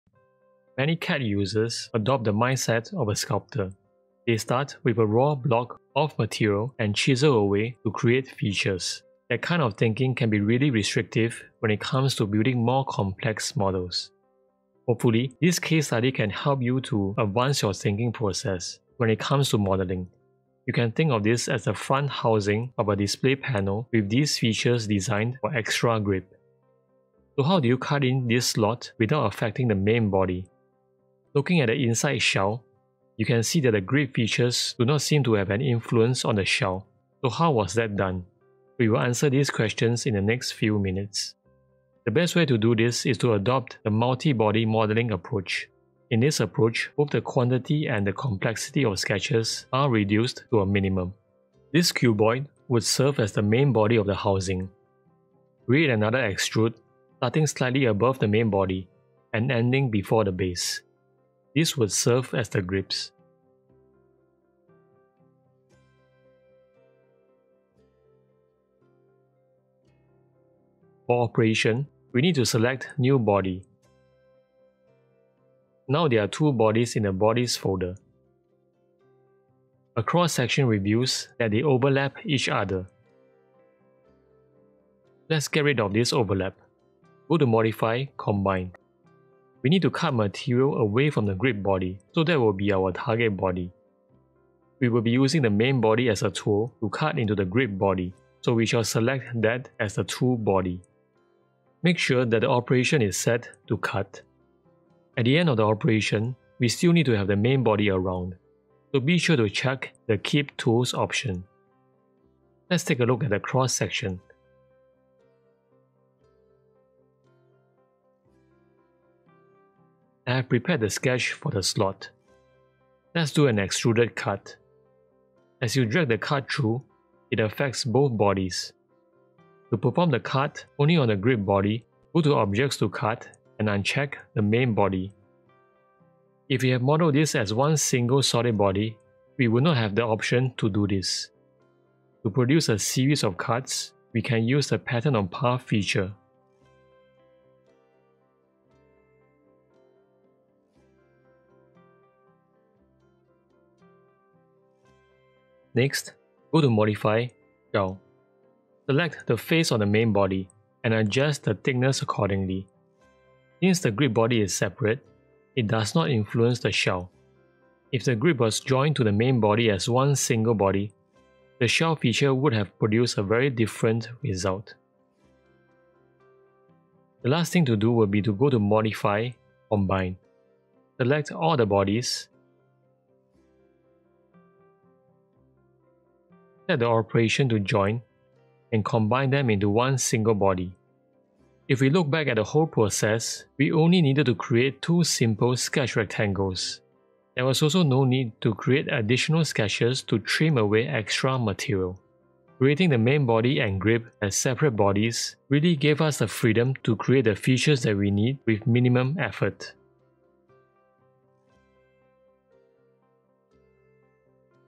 Fusion 360 - Case Study #2
In this case study, we will be looking at using the multi-body strategy.